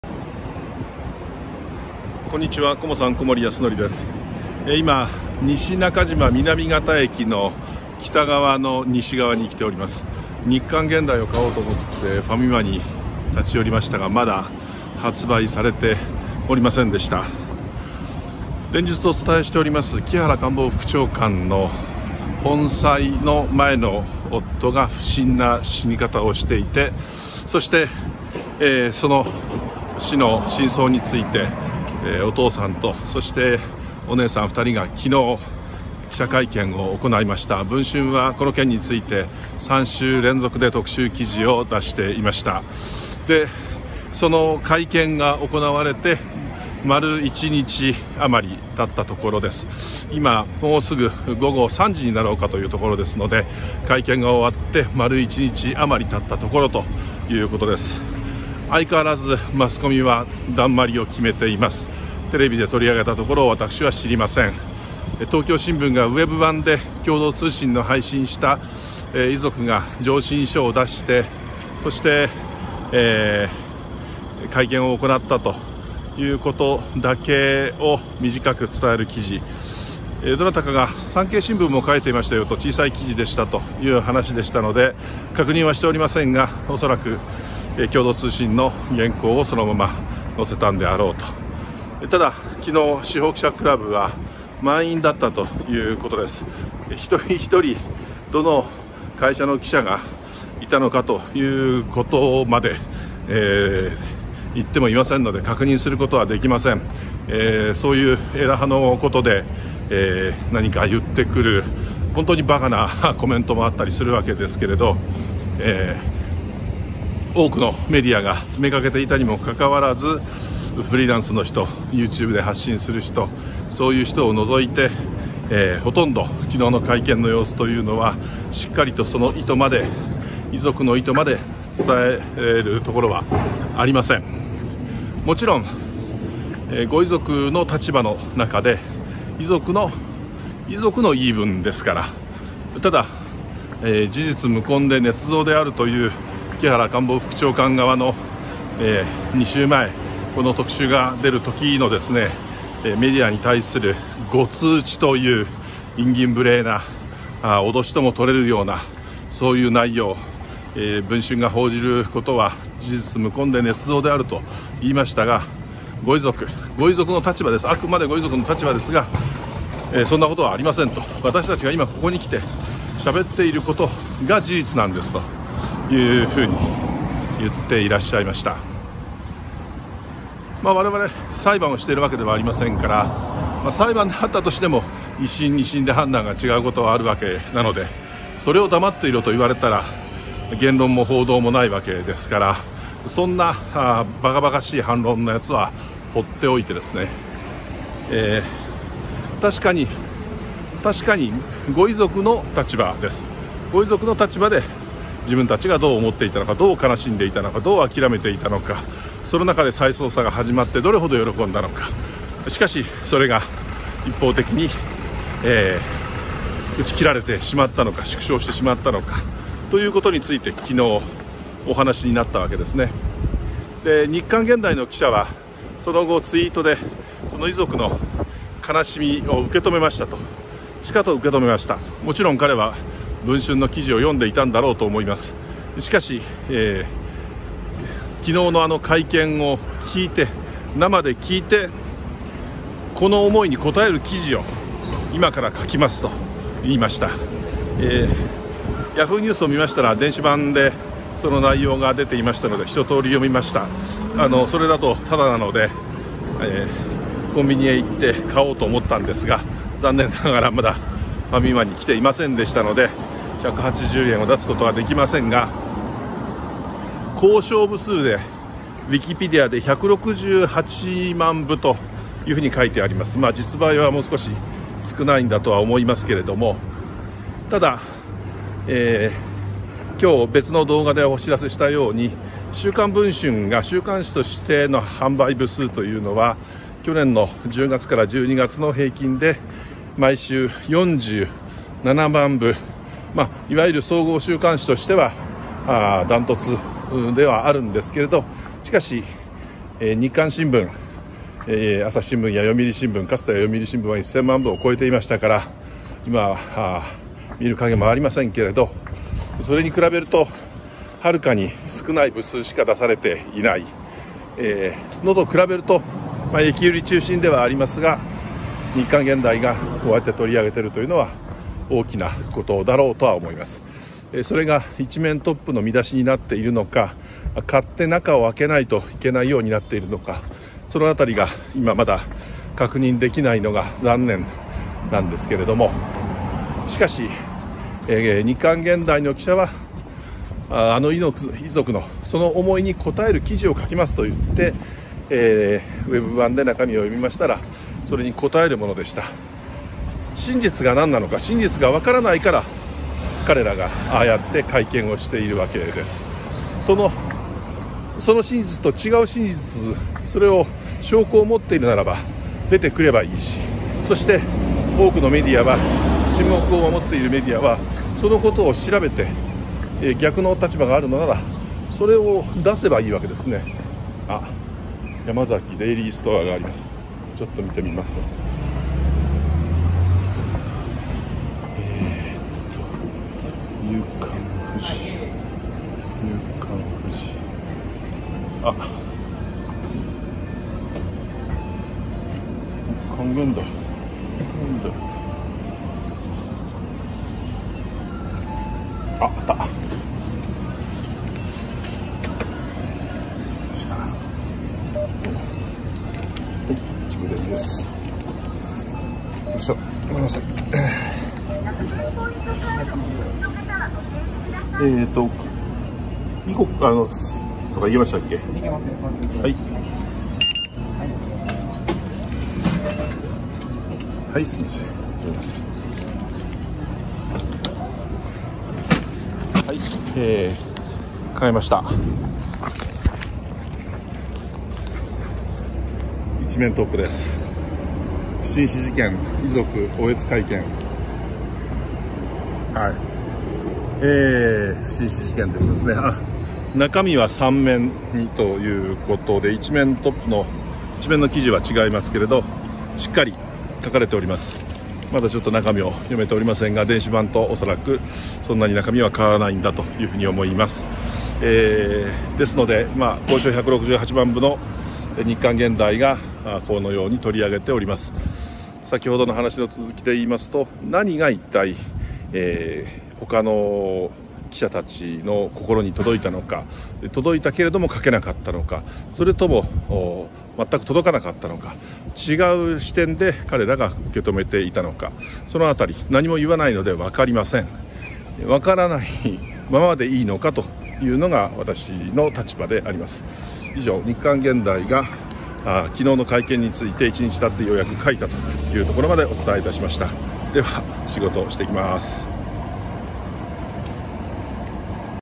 木原問題 日刊ゲンダイが取り上げました！ 週刊文春単独砲に援軍
昨日の遺族記者会見を日刊ゲンダイが報じました。ネット版で記事を見たので、西中島南方駅前のコンビニで買おうとしましたが、まだ届いていませんでした。

記者会見で語られたのはあくまで遺族側の視点です。しかし「真実を知りたい」という強い思いと、再捜査への感謝、落胆、文春報道への期待などは伝わりました。日刊ゲンダイの記者は、その思いを受けて記事を書きました。

同じように会見を聞いた他社の記者は会見後に記事を書いたのでしょうか？　出稿したけど没になったのでしょうか？　朝日新聞や毎日新聞、読売新聞、テレビ各社が取り上げないのでサッパリ理由が分かりません。

話しながら歩いていると、デイリーヤマザキがあったので日刊ゲンダイを買いました。１面に見出しが踊っています。

「不審死」事件　遺族嗚咽会見　でした。